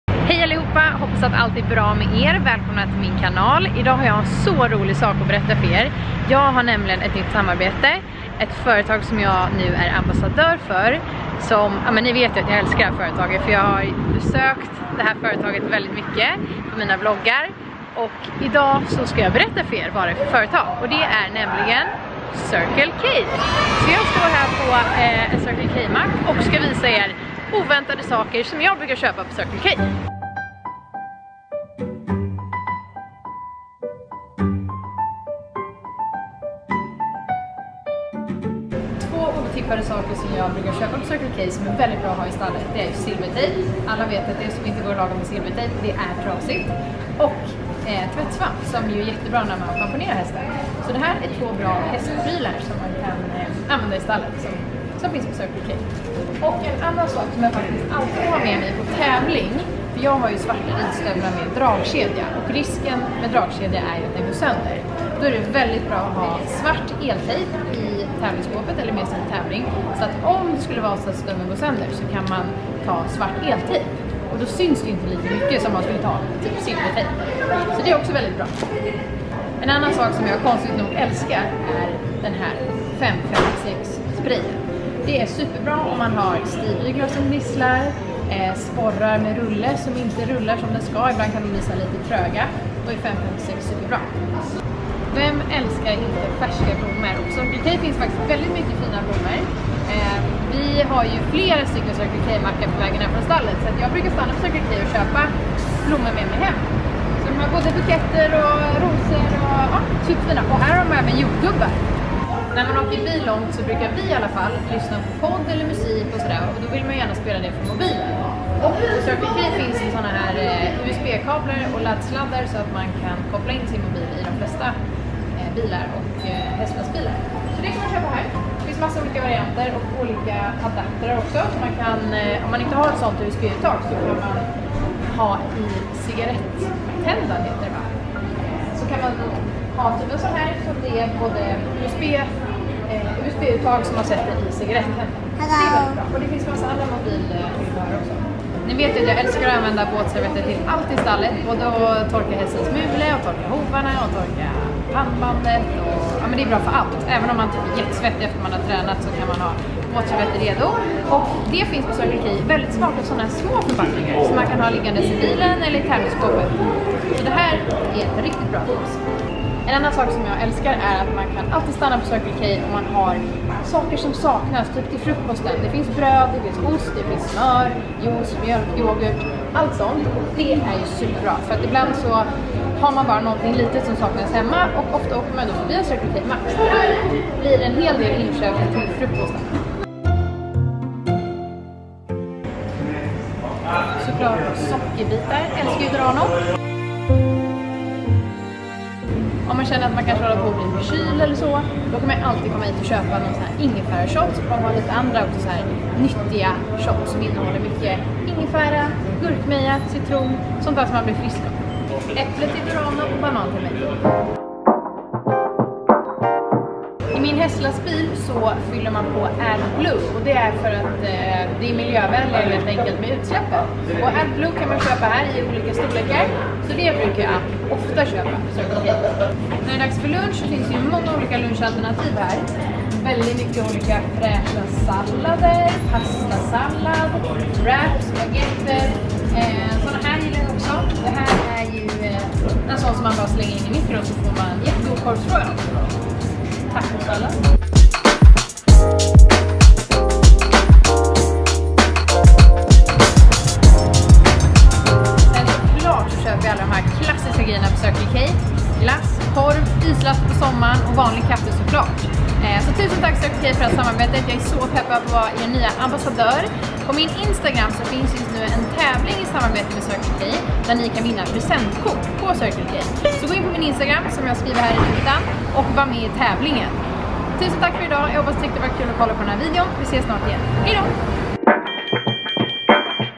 STOR NYHET! 😀🐴❤️
Videon är i samarbete med Circle K Sverige.

ÄNTLIGEN kan jag berätta för er att jag är ny ambassadör för Circle K!!! Så otroligt roligt! Circle K har alltid varit mitt favoritstopp längsmed vägarna för att tanka energi, både i bilen och mig själv. Det känns därför som en självklar samarbetspartner till mig och jag är verkligen en stolt ambassadör. För att fira detta tävlar jag just nu ut 3 presentkort värde 1000 kr styck på min instagram, följ mig där @ninasrader och var med i tävlingen :)

Här kommer även en video där jag visar er lite otippade saker som jag brukar köpa på Circle K, flera saker som är väldigt användbara i stallet! Hoppas ni gillar videon och är lika taggade på detta som jag är!